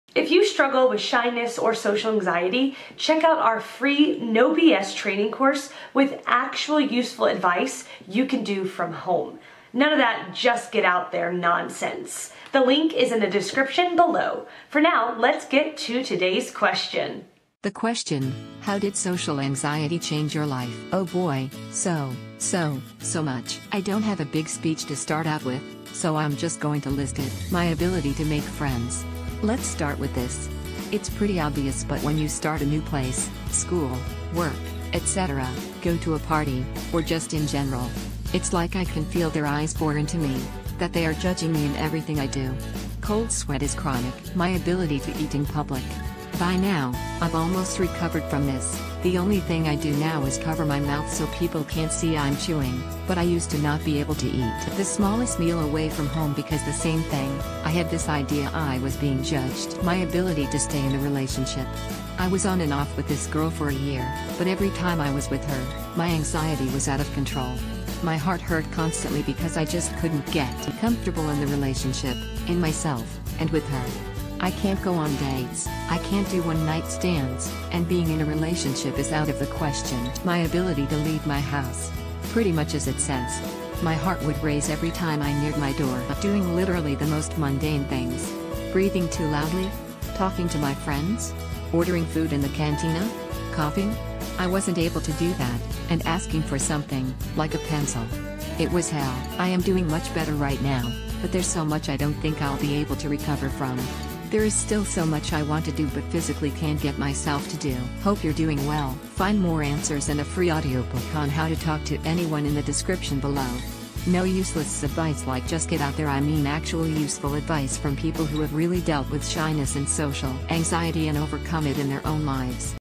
How Did Social Anxiety Change Your Life?
My ability to make friends. Let's start with this. It's pretty 'obvious' but when you start a new place (school, work, etc.), go to a party, or just in general. It's like I can feel their eyes bore into me,